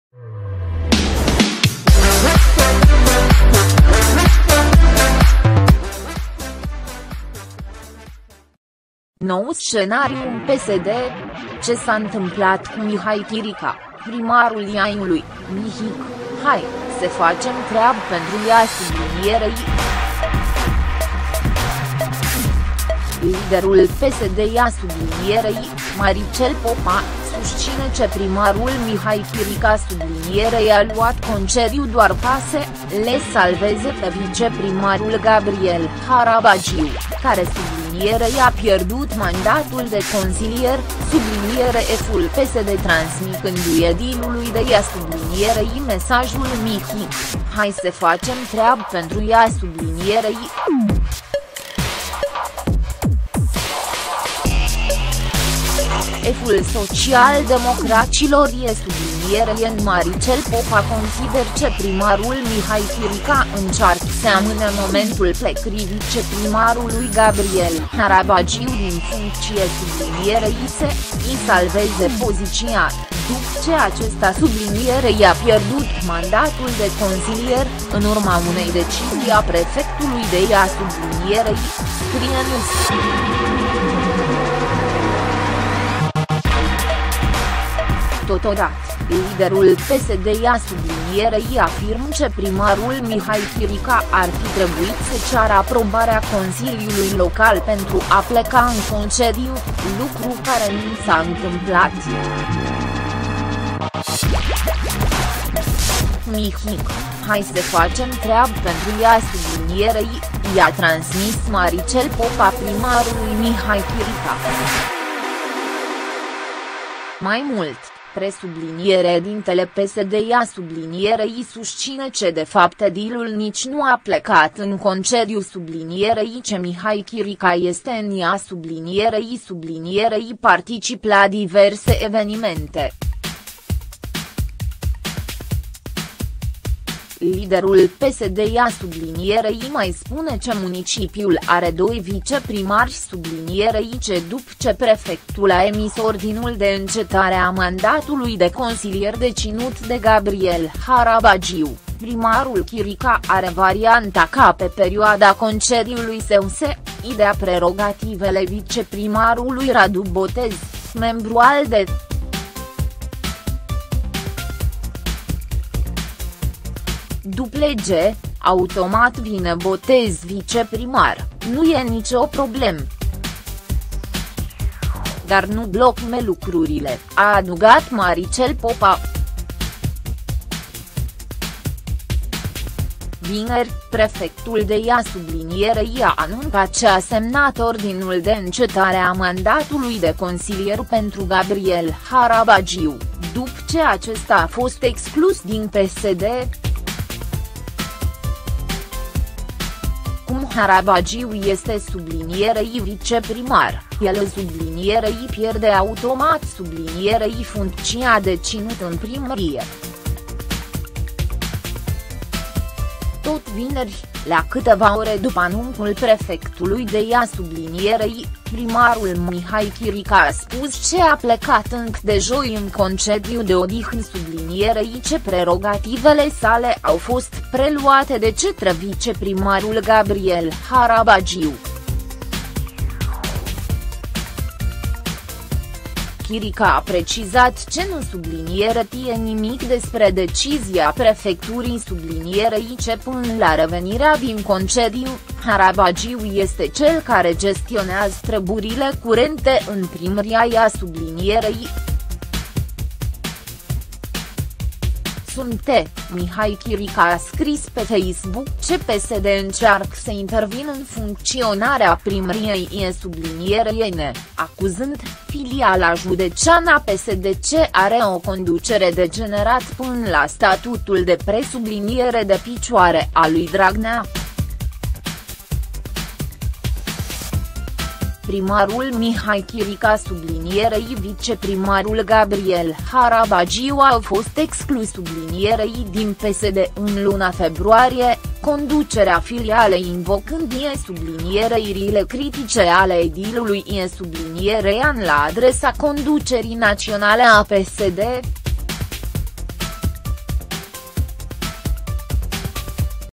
Nou SCENARIU în PSD! Ce s-a întâmplat cu Mihai Chirica, primarul Iașiului: 'Mihăiţă, hai
Nou SCENARIU în PSD! Ce s-a întâmplat cu Mihai Chirica, primarul Iașiului: 'Mihăiţă, hai, să facem treabă pentru Iaşi!'